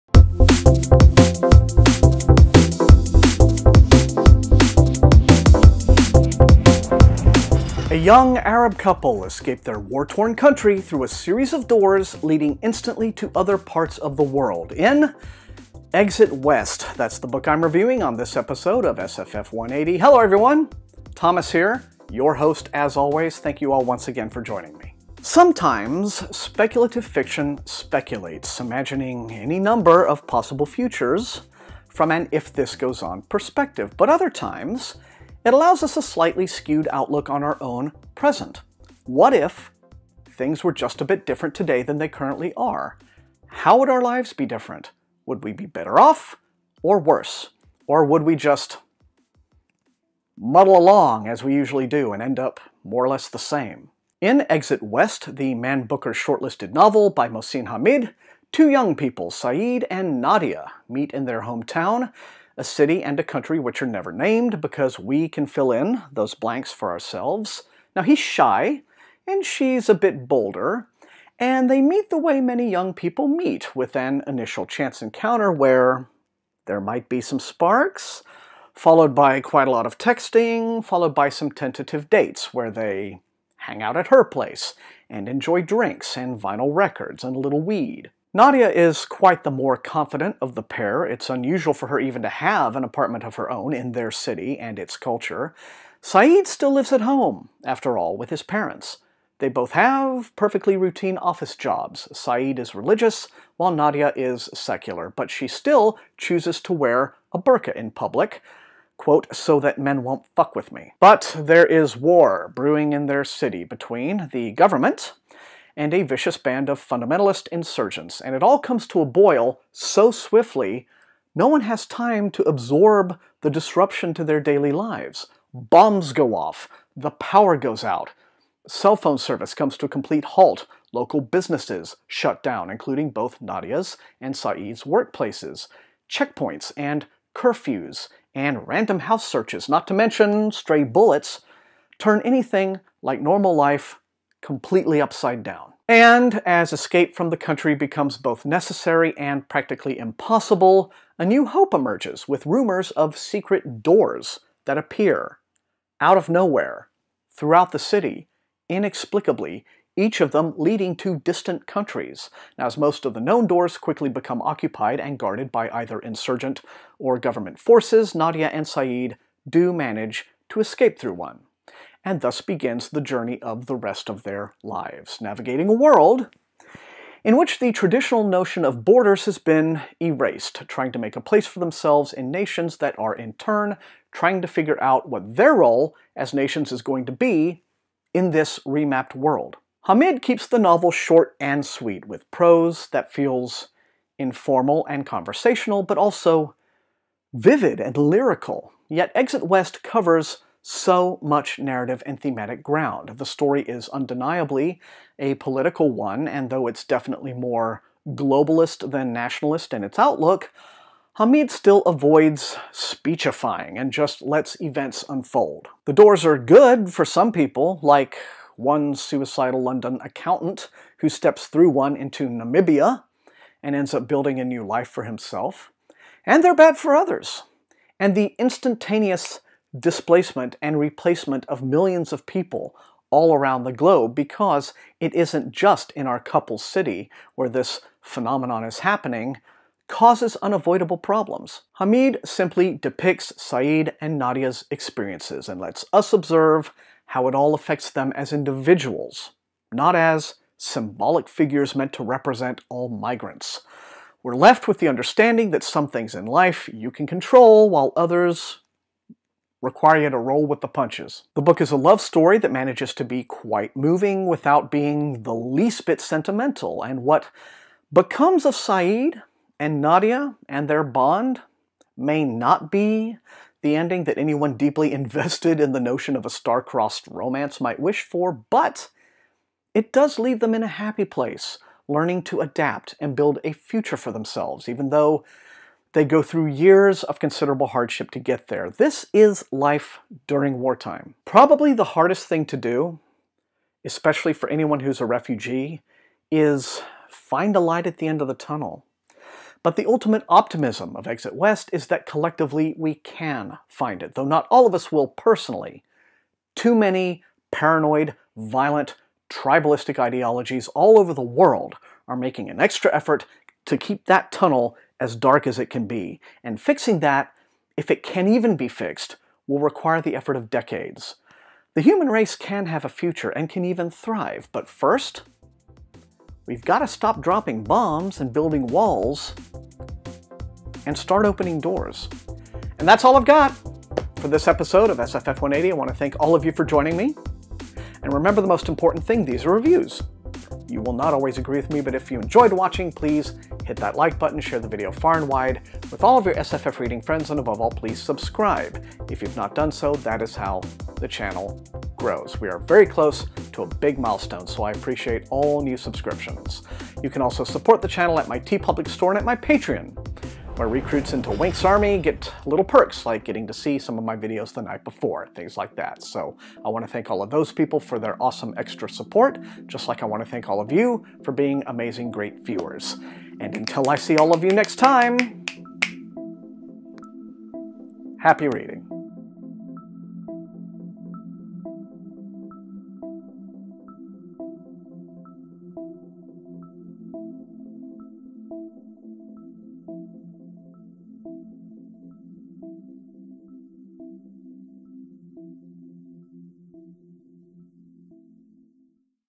SFF180 🌍 ‘Exit West’ by Mohsin Hamid ★★★★
A young Arab couple escape their war-torn country through a series of doors leading instantly to other parts of the world in this Man Booker Prize finalist.

Support SFF180!